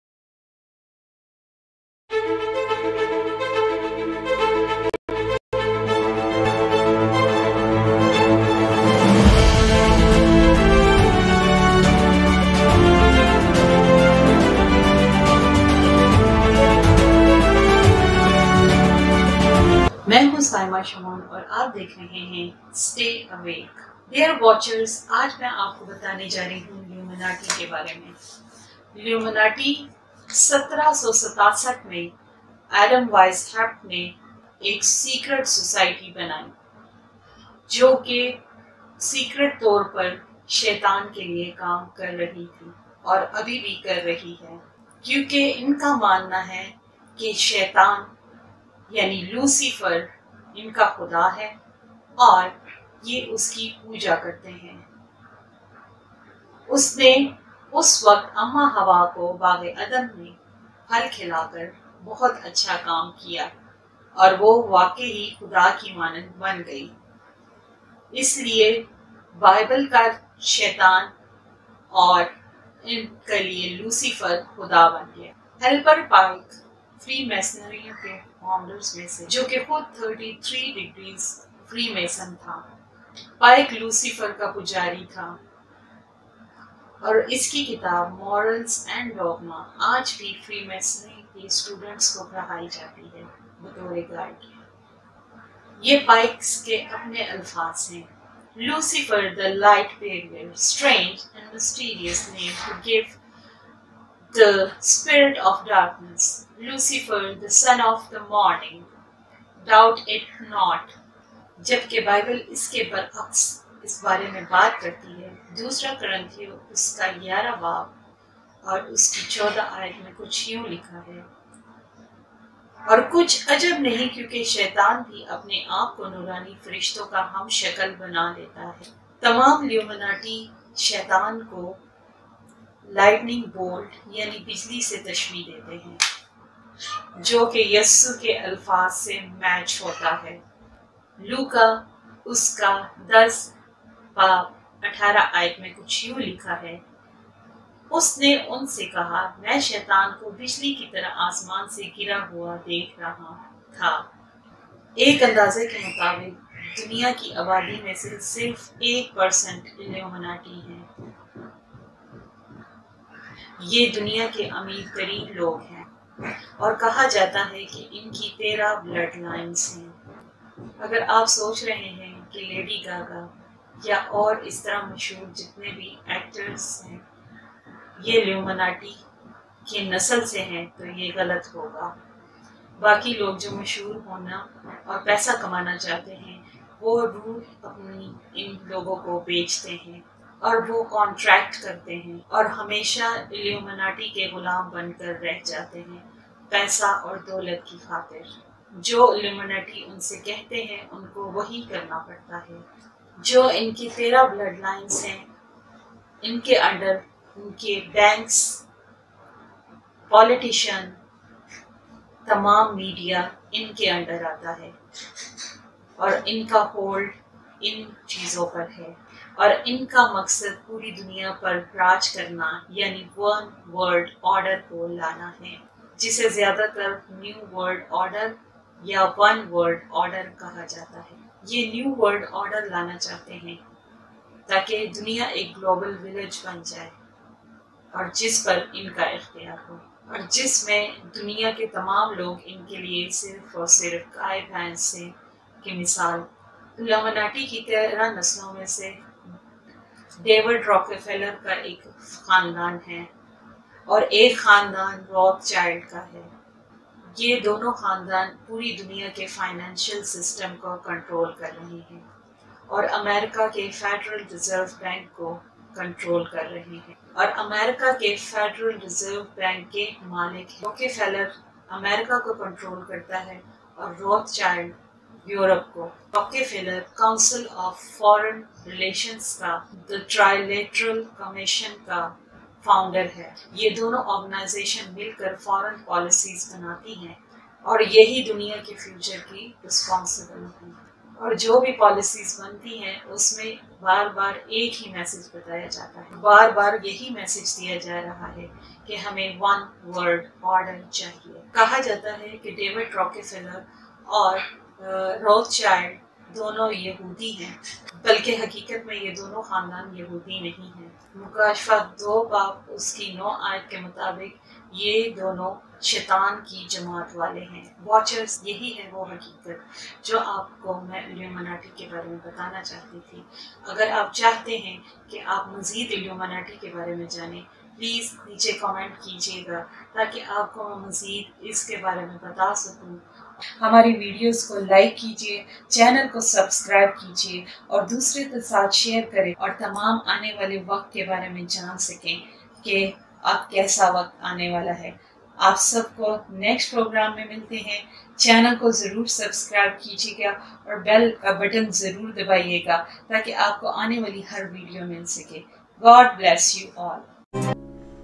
Stay Awake [Jagty Raho] || Illuminati Exposed || Episode 1 || Divine Faith TV
Stay Awake [Jagty Raho] is an informative program to educate our new generation about the new world order agenda and Illuminati.